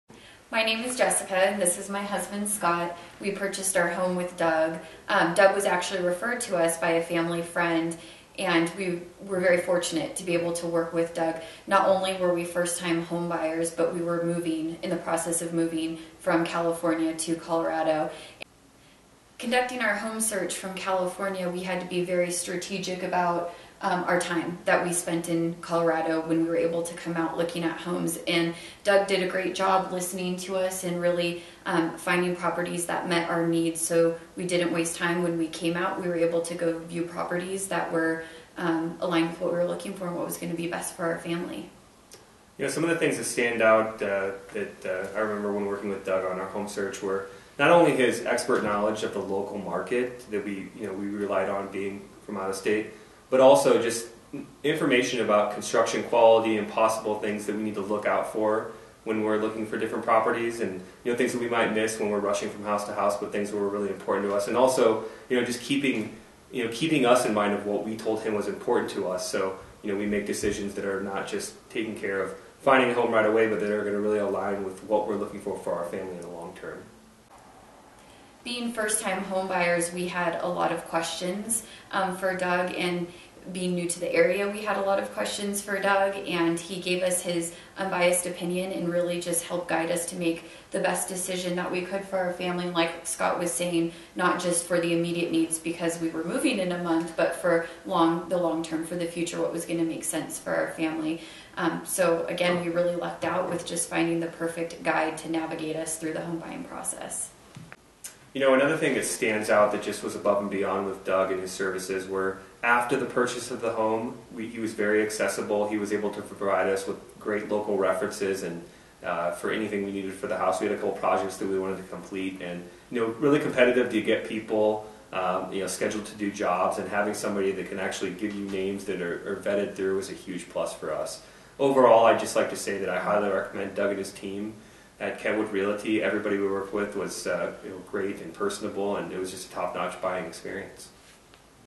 First Time Buyer Testimonial - Jessica and Scott
Jessica and Scott are first time home buyers that relocated from out of state. They talk about the services Doug and his team provided in helping them find a great home in Castle Pines and the continued service provided after Closing.